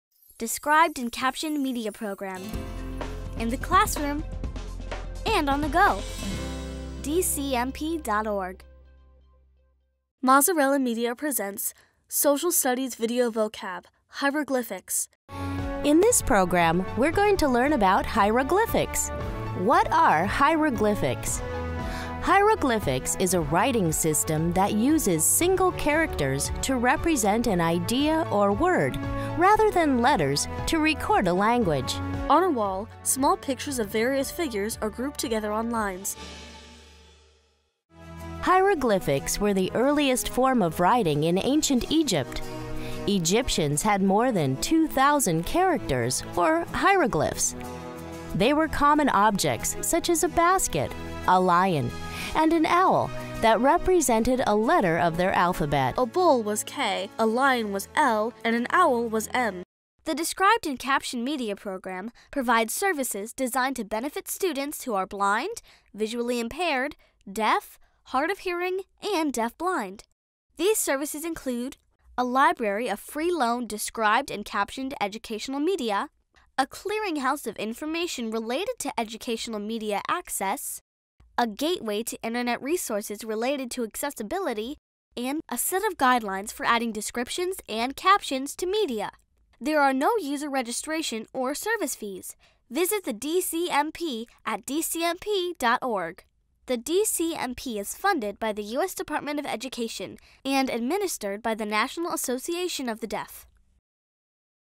Social Studies Video Vocab: Hieroglyphics (Accessible Preview)
Features a wide variety of video footage, photographs, diagrams, graphics, and labels. Begins with a simple definition which helps clarify pronunciation and provides opportunities to transfer words from working to long-term memory. Also concludes with a critical thinking question. For this particular clip, students will focus on the term hieroglyphics.

Producer/Distributor: MAZZARELLA MEDIA
Production Year: 2013
Grade Level: 4-8